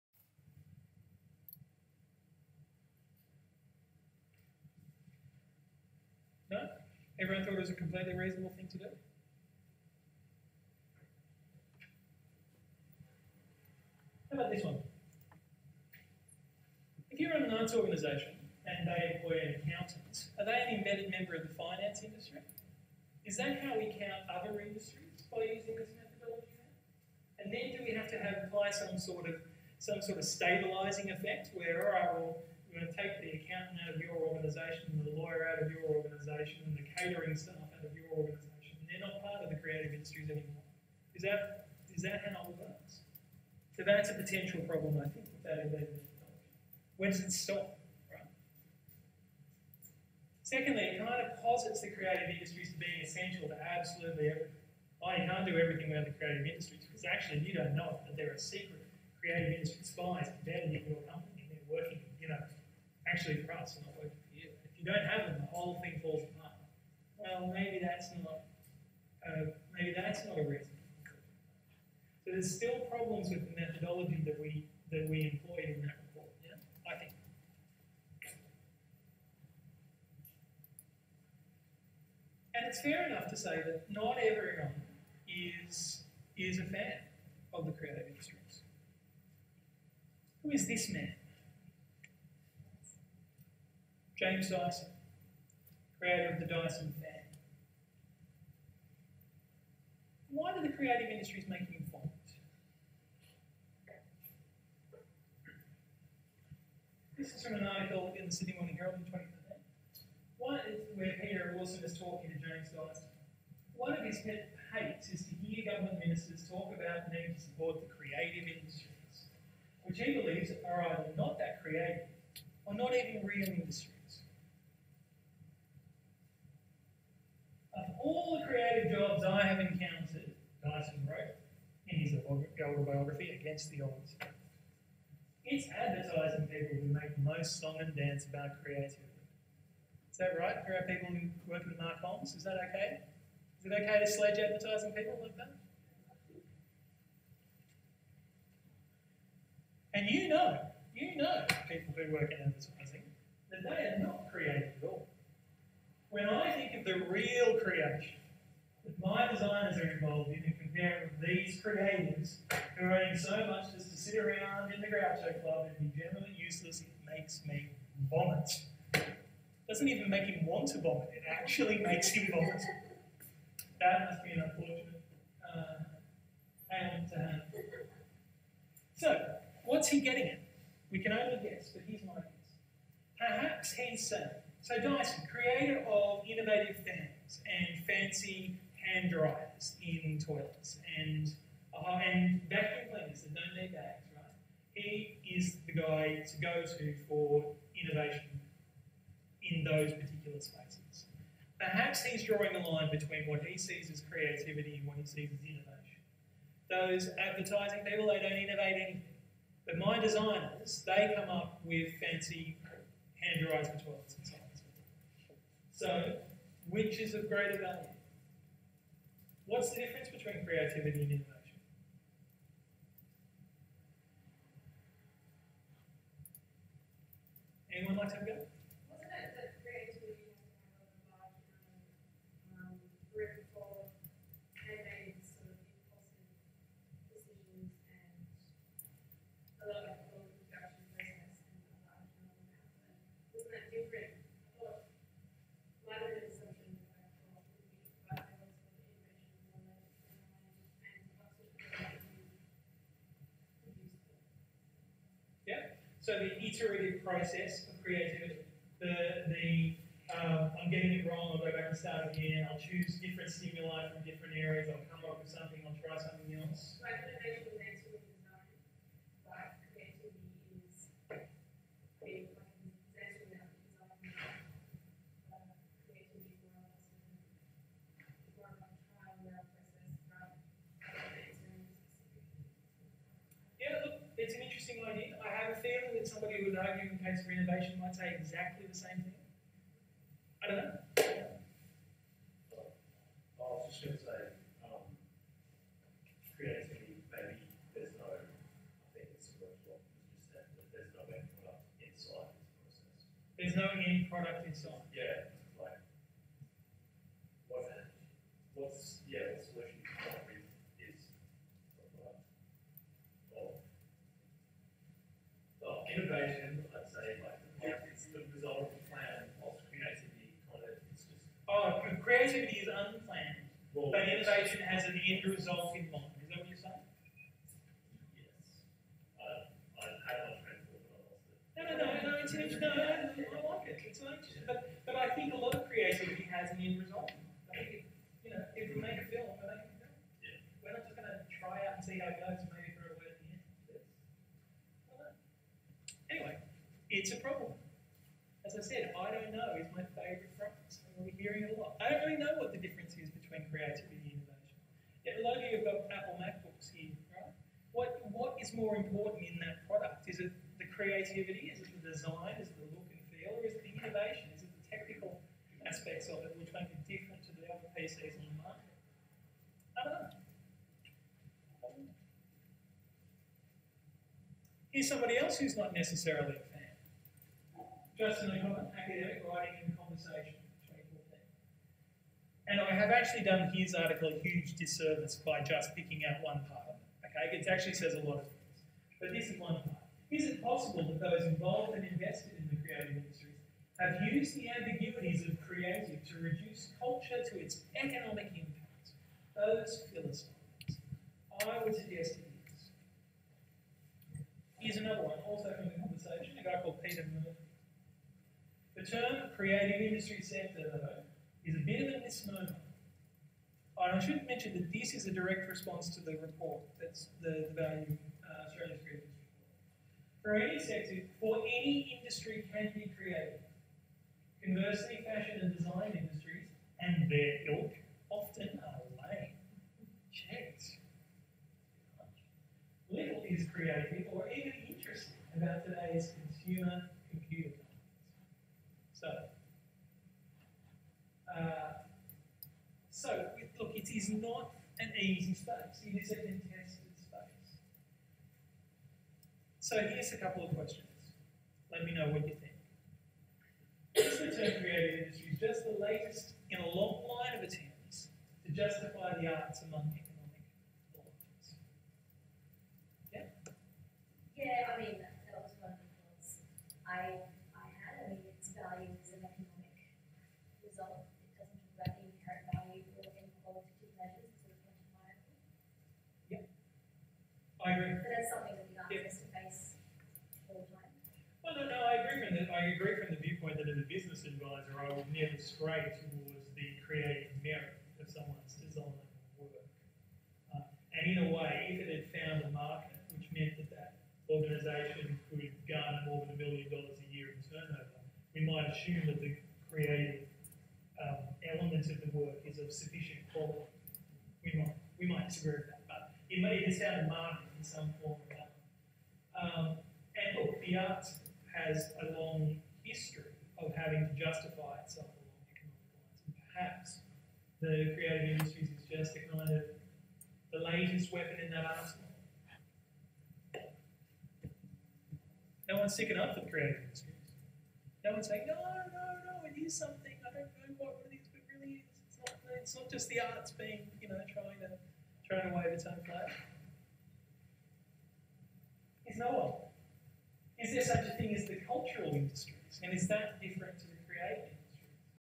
David Sharpe on embedded creatives
guest lecture on Tuesday 3rd March 2015